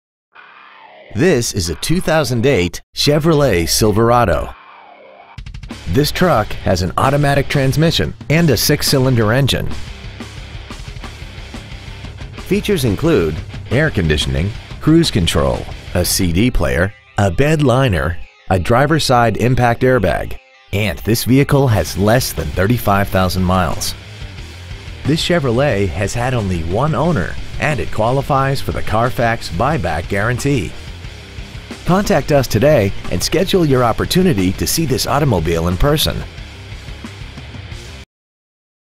2008 Chevrolet Silverado Chandler AZ 85225
Year : 2008
 Make : Chevrolet
 Model : Silverado
 Engine : 4.3L 6 Cyl. Fuel Injected
 Trans . : Automatic
 Exterior : Blue
 Miles : 34,484

 Stock : 1157621

 2121 N. Arizona Avenue